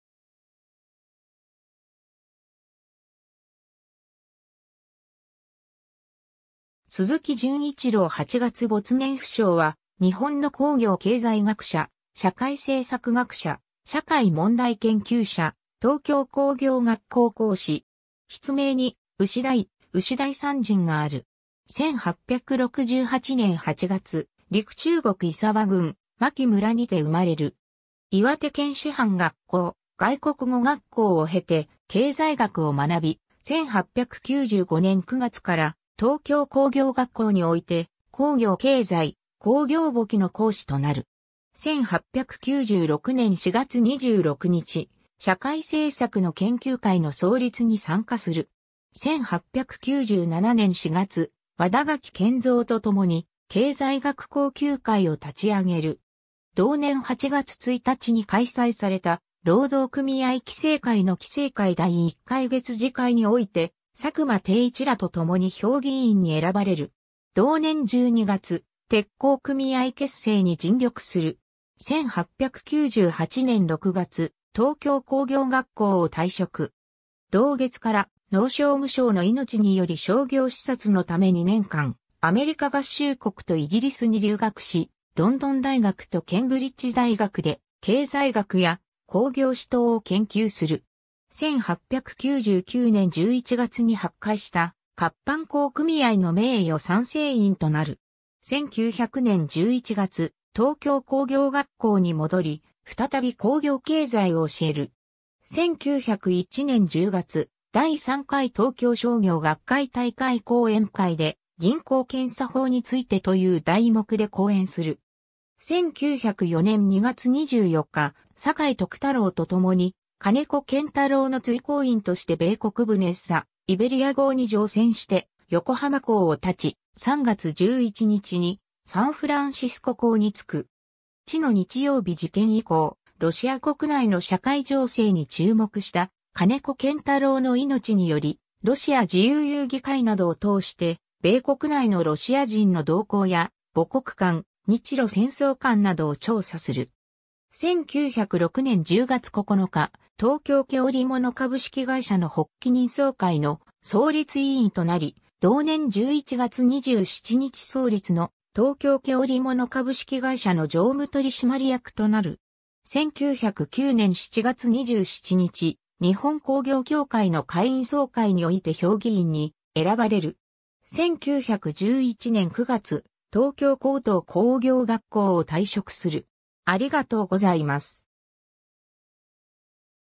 鈴木純一郎
鈴木 純一郎（すずき じゅんいちろう、1868年（明治元年）8月 - 没年不詳）は、日本の工業経済学者、社会政策学者、社会問題研究者、東京（高等）工業学校講師。
筆名に牛台、牛台山人がある。
 1868年（明治元年）8月、陸中国胆沢郡真城村にて生まれる。
 岩手県師範学校、外国語学校を経て経済学を学び、1895年（明治28年）9月から東京工業学校において工業経済、工業簿記の講師となる。
 1896年（明治29年）4月26日、社会政策の研究会（後の「社会政策学会」）の創立に参加する。
 1897年（明治30年）4月、和田垣謙三と共に経済学攻究会を立ち上げる。
同年8月1日に開催された労働組合期成会の期成会第一回月次会において佐久間貞一らと共に評議員に選ばれる。
同年12月、鉄工組合結成に尽力する。
 1898年（明治31年）6月、東京工業学校を退職。
同月から農商務省の命により商業視察のため2年間アメリカ合衆国とイギリスに留学し、ロンドン大学とケンブリッジ大学で経済学や工業史等を研究する。
 1899年（明治32年）11月に発会した活版工組合の名誉賛成員となる。
 1900年（明治33年）11月、東京工業学校に戻り、再び工業経済を教える。
 1901年（明治34年）10月、第3回東京商業学会大会講演会で「銀行検査法に就て」という題目で講演する。
 1904年（明治37年）2月24日、阪井徳太郎と共に金子堅太郎の随行員として米国船サイベリア号に乗船して横浜港を発ち、3月11日にサンフランシスコ港に着く。
血の日曜日事件以降、ロシア国内の社会情勢に注目した金子堅太郎の命により、ロシア自由友誼会などを通して米国内のロシア人の動向や母国観、日露戦争観などを調査する。
 1906年（明治39年）10月9日、東京毛織物株式会社の発企人総会の創立委員となり、同年11月27日創立の東京毛織物株式会社の常務取締役となる。
 1909年（明治42年）7月27日、日本工業協会の会員総会において評議員に選ばれる。
 1911年（明治44年）9月、東京高等工業学校を退職する。